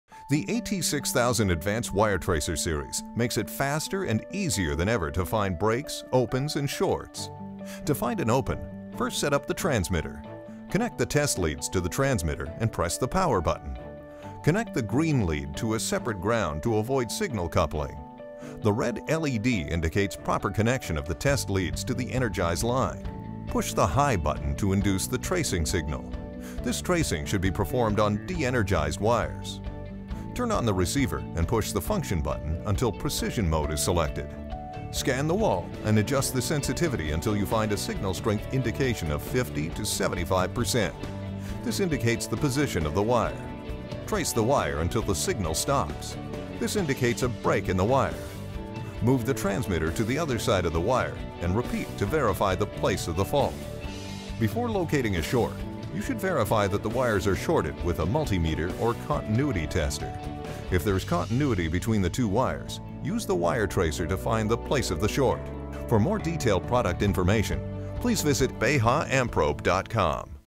How to find break, opens and shorts in wires (subtitles available: FI/NO/PL/SW)
How to find break, opens and shorts with the
Beha-Amprobe AT-6000-EUR Series Advanced Wire Tracer.